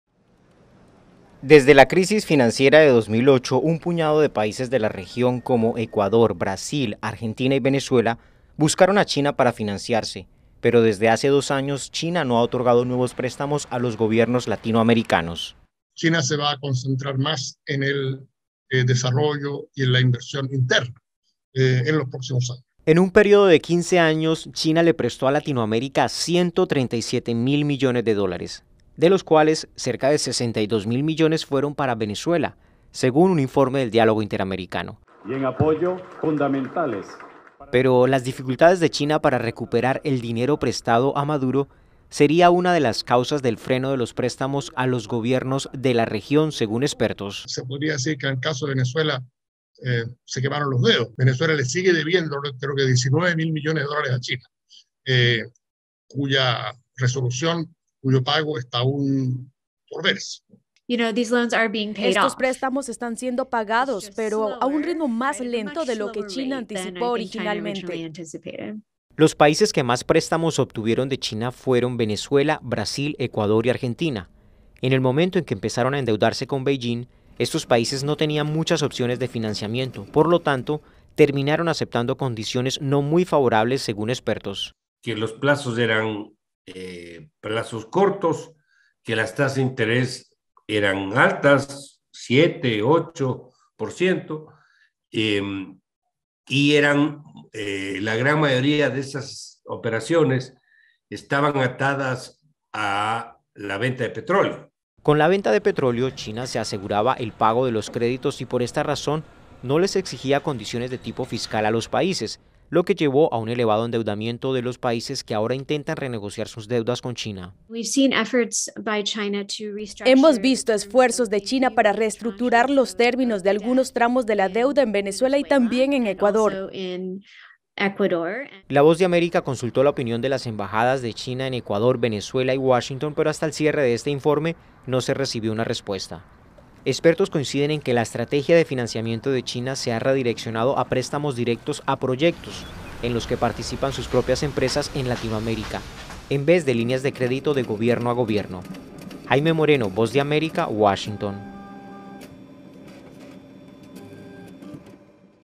Préstamos de China en pausa para gobiernos de Latinoamérica
Desde 2020 China no ha emitido nuevos créditos a los gobiernos de la región. Expertos aseguran que la incertidumbre por el pago de la deuda de Venezuela, entre otros factores, explican la postura prudente adoptada por China sobre otorgar préstamos de gobierno a gobierno en Latinoamérica.

¡Visítenos!